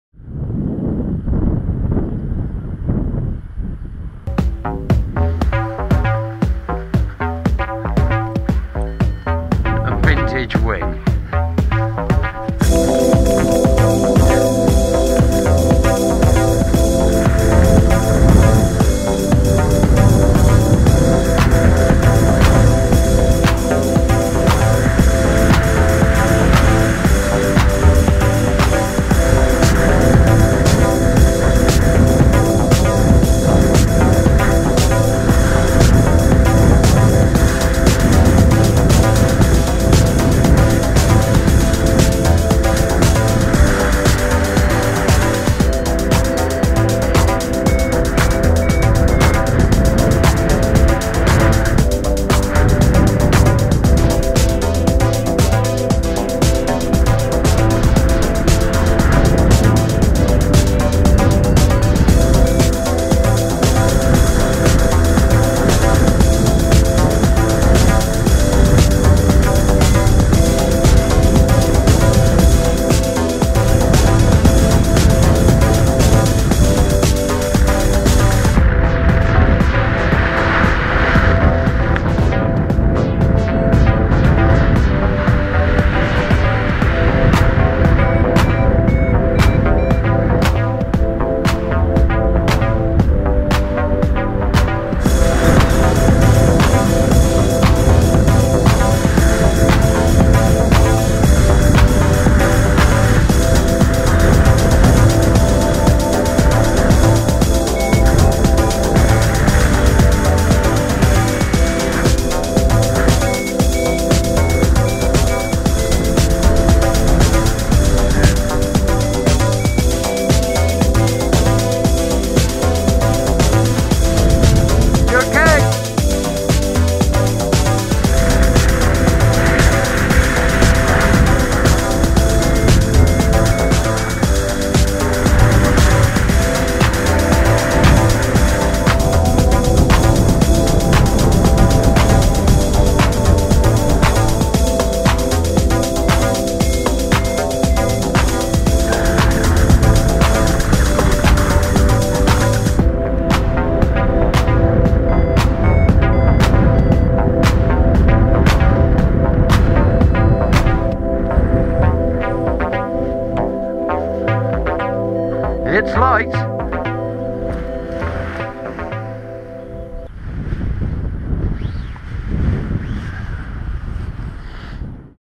Paragliding Scratching the horse
7th of March, a scratch around the white horse at Westbury, nothing epic. I arrived a bit late and missed the stronger lift. Not to worry I was quite happy to have decent half hour of ground rushes on my trusty sweet handling old Nova Factor winter hack.